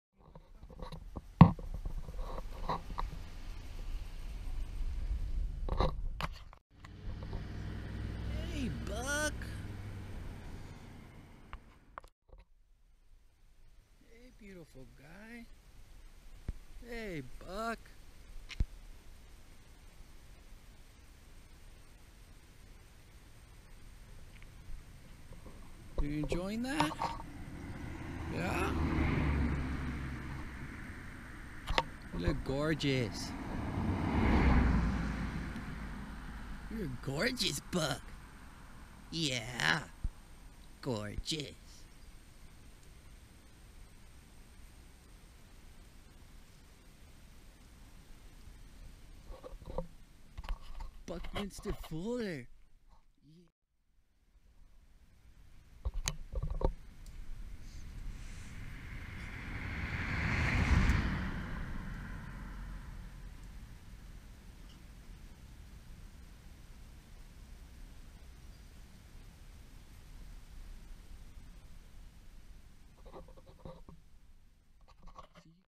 Catching Two Bucks Grazing on the Side of the Road in Yosemite Valley - National Parks
We ran into two bucks grazing off the side of the road while driving through the Yosemite Valley Loop.